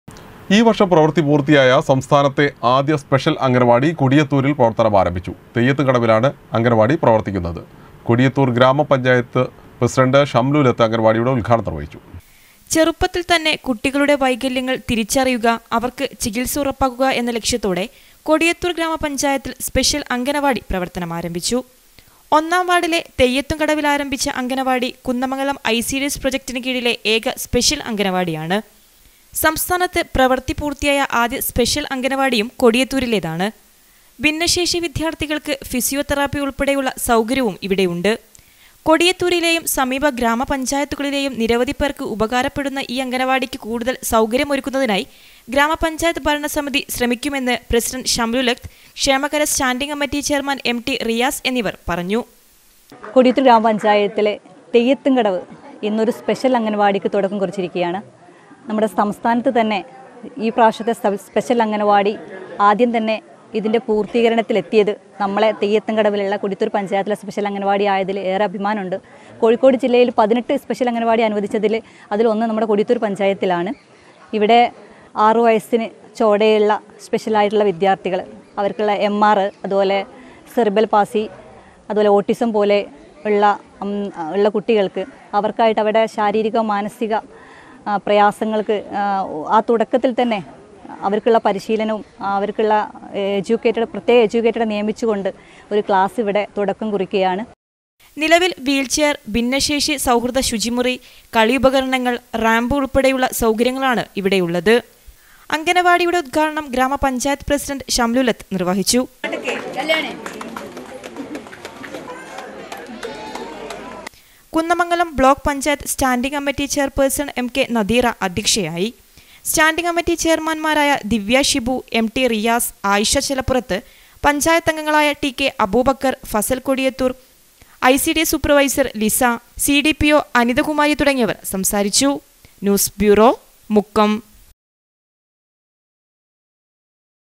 ഈ വർഷം പ്രവൃത്തി പൂർത്തിയായ സംസ്ഥാനത്തെ ആദ്യ സ്പെഷ്യൽ അംഗൻവാടി കൊടിയത്തൂരിൽ പ്രവർത്തനമാരംഭിച്ചു
ഈ വർഷം പ്രവൃത്തി പൂർത്തിയായ സംസ്ഥാനത്തെ ആദ്യ സ്പെഷ്യൽ അംഗൻവാടി കൊടിയത്തൂരിൽ പ്രവർത്തനമാരംഭിച്ചു. തെയ്യത്തും കടവിലാണ് അംഗൻവാടി പ്രവർത്തിക്കുന്നത്. കൊടിയത്തൂർ ഗ്രാമപഞ്ചായത്ത് പ്രസിഡൻ്റ് ഷംലൂലത്ത് അംഗൻവാടിയുടെ ഉദ്ഘാടനം നിർവഹിച്ചു